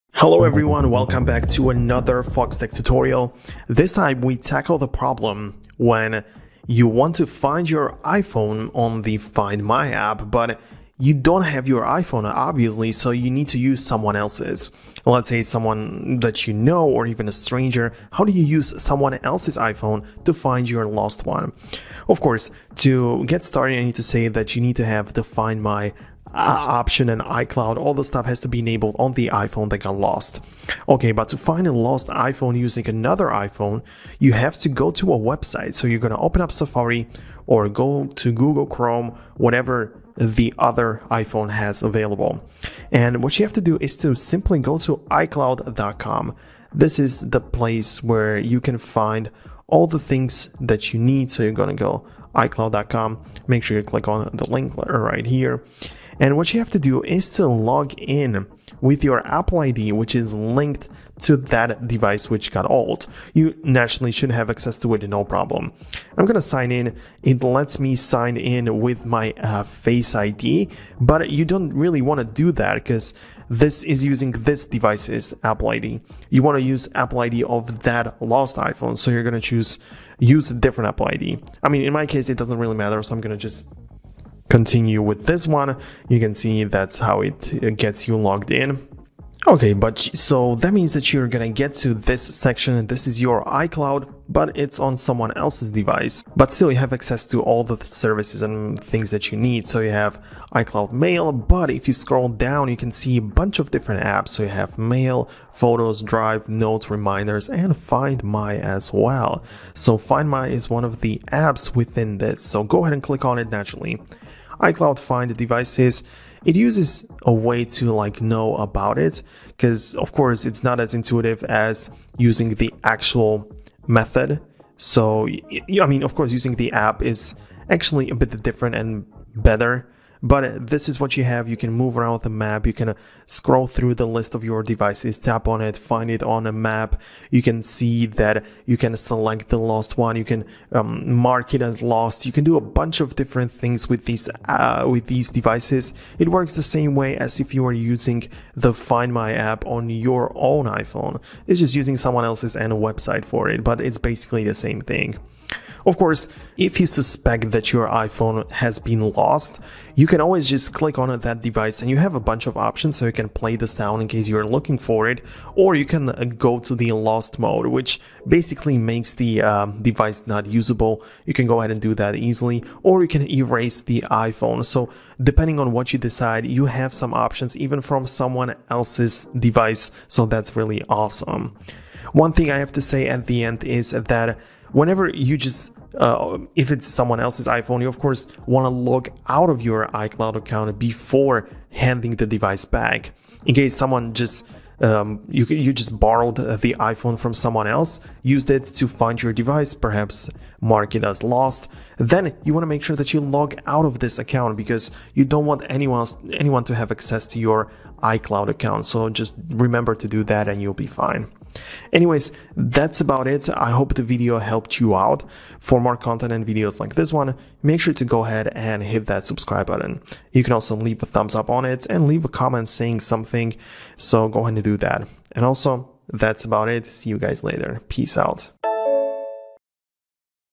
How to Find my iPhone from Another iPhone (tutorial)
Misplaced your iPhone and need to find it fast? This tutorial demonstrates the simple steps to track and locate your lost iPhone using another iOS device. Discover how to utilize Apple's Find My app to pinpoint your device's exact location and take swift action. Don't panic – watch this guide to reunite with your iPhone in no time!

Timestamps:
0:00 Find My iPhone from Another Device
0:23 iCloud.com
1:33 Find iPhone
3:24 Conclusion